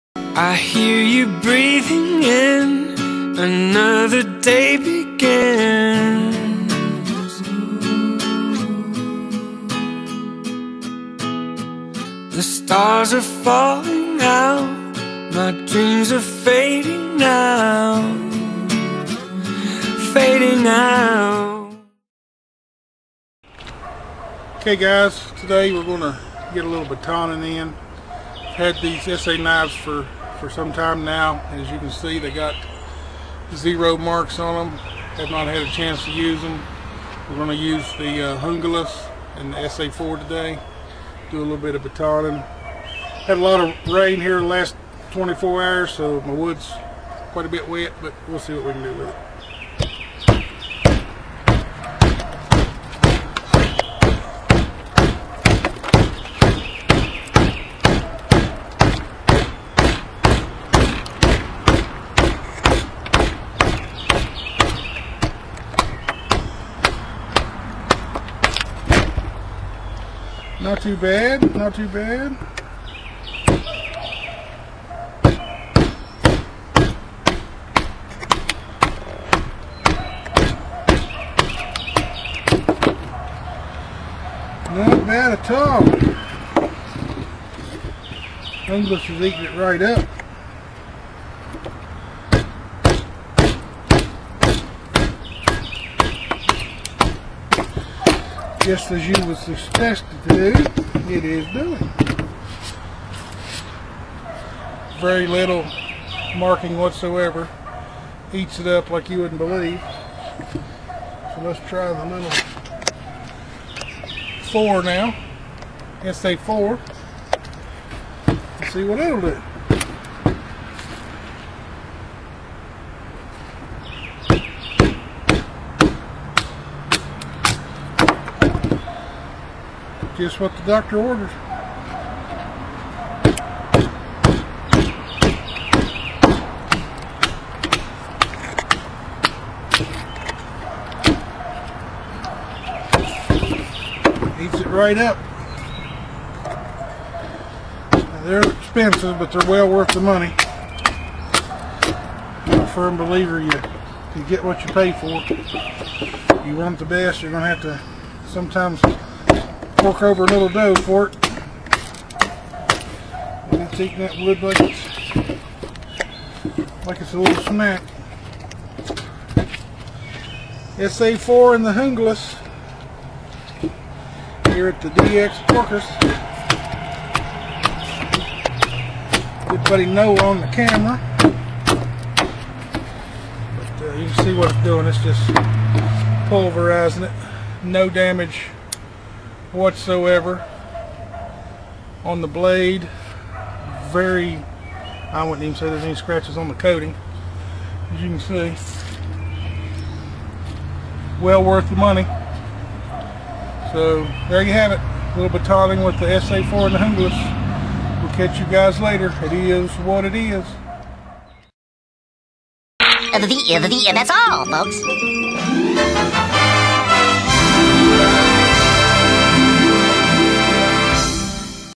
Batoning with ESEE
Some Batoning with some ESEE Products. ESEE 4 and the Junglas. I am really liking the ESEE stuff. More videos to follow.